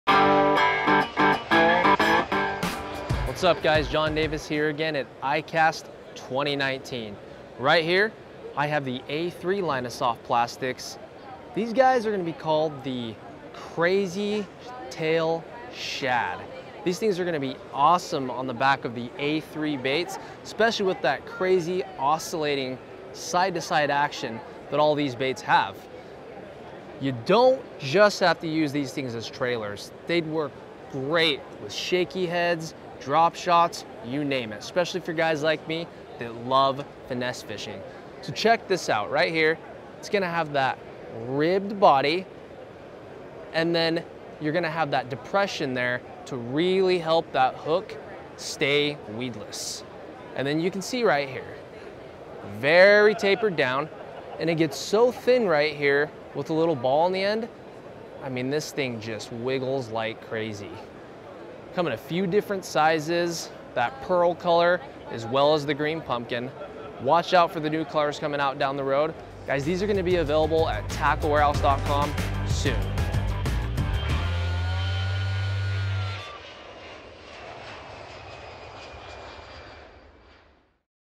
A3 Anglers Crazy Tail Shad with Jon Davis | iCast 2019
TW discusses the A3 Anglers Crazy Tail Shad with Jon Davis at the 2019 ICAST Show in Orlando, Florida.